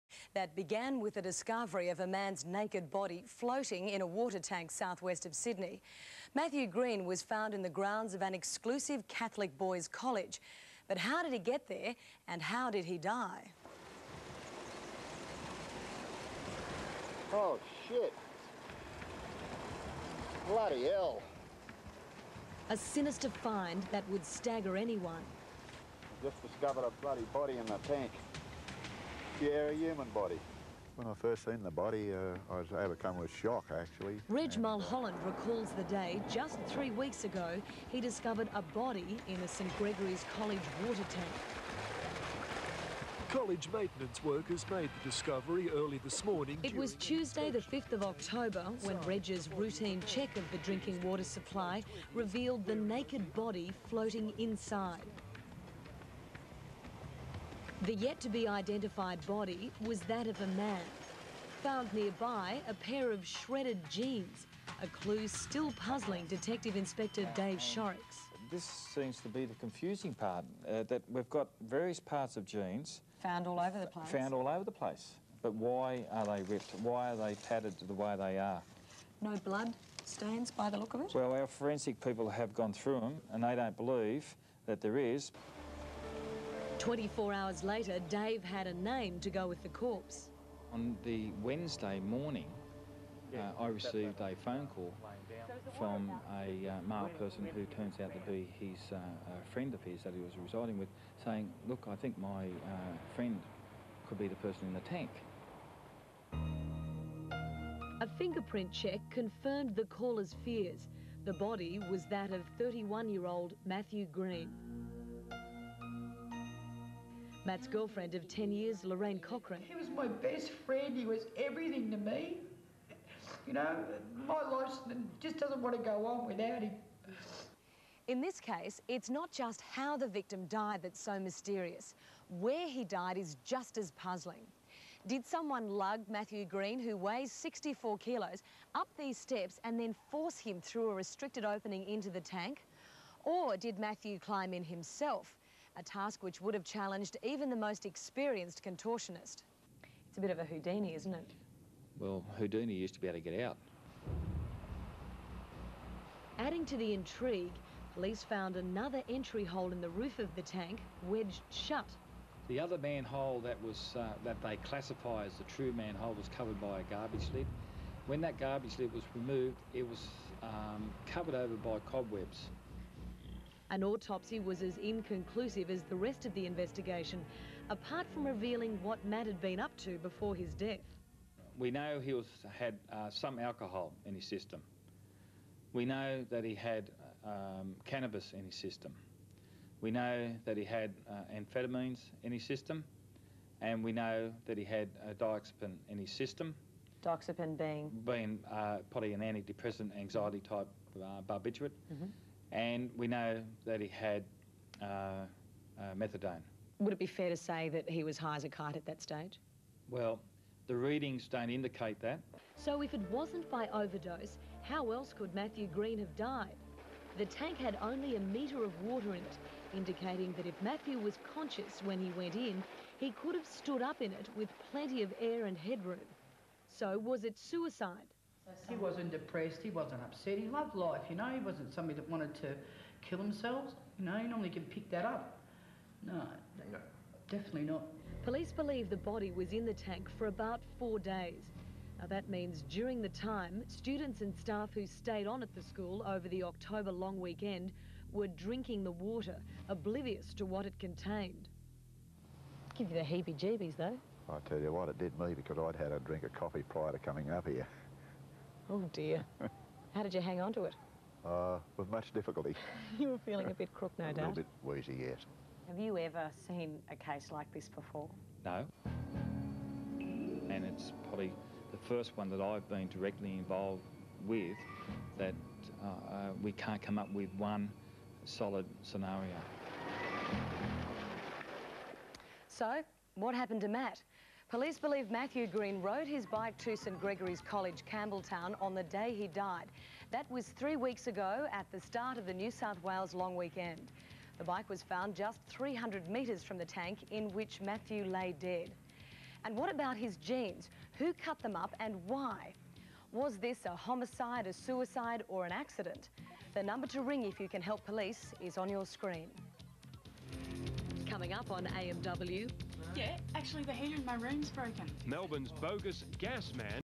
My Brother Matt
My brother has passed on without leaving us much to go on regarding his death